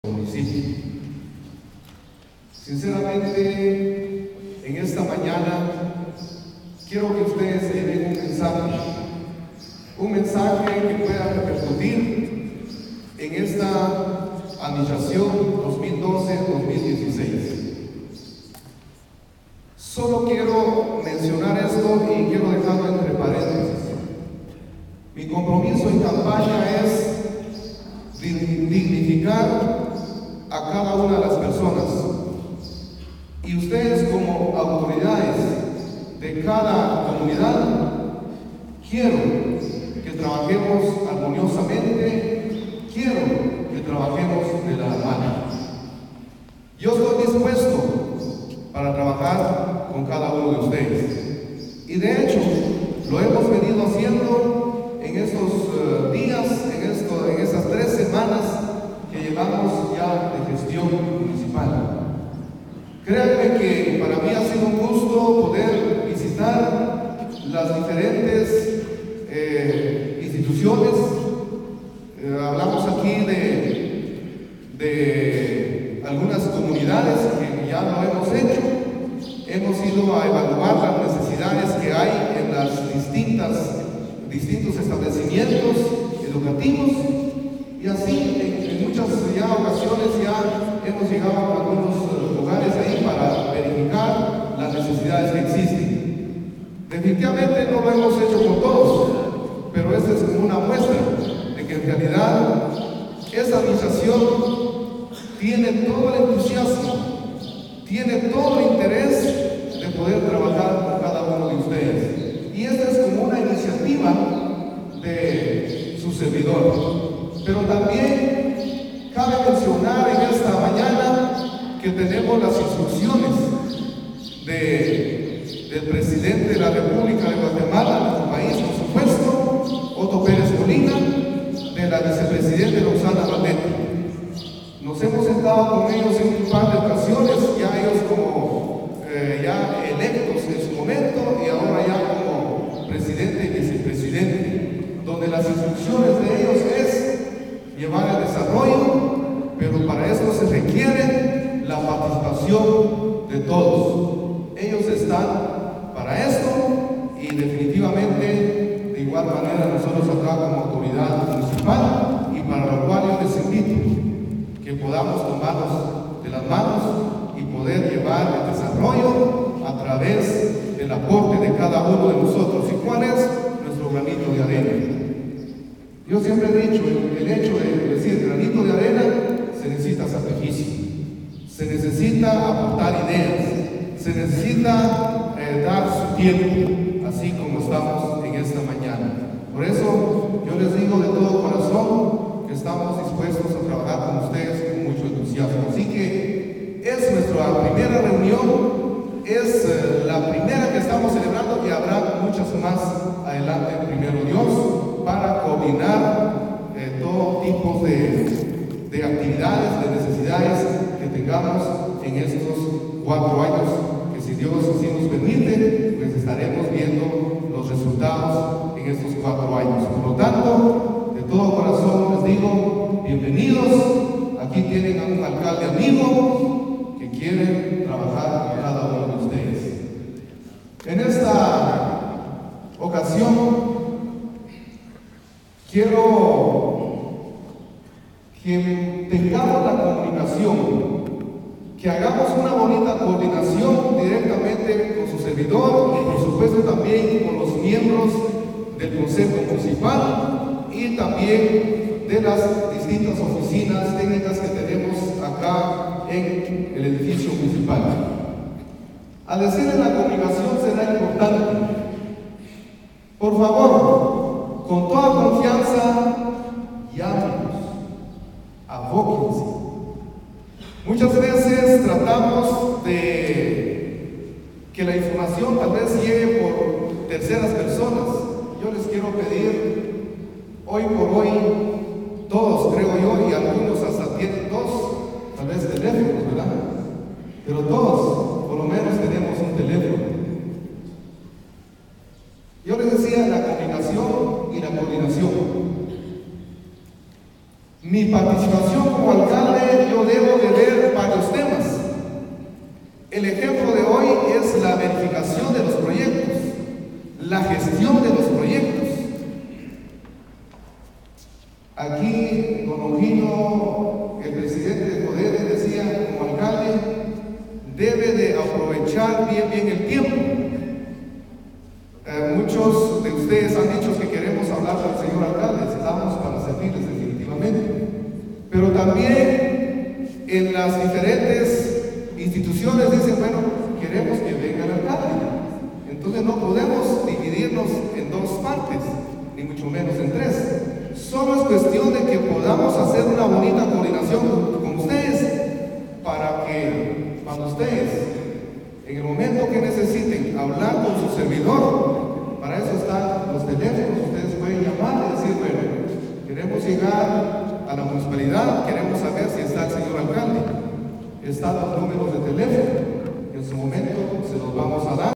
1ª reunión de Autoridades locales
Patzún, 1ª reunión de Autoridades locales, presentación del Alcalde Mardoqueo Cancax, 1ª parte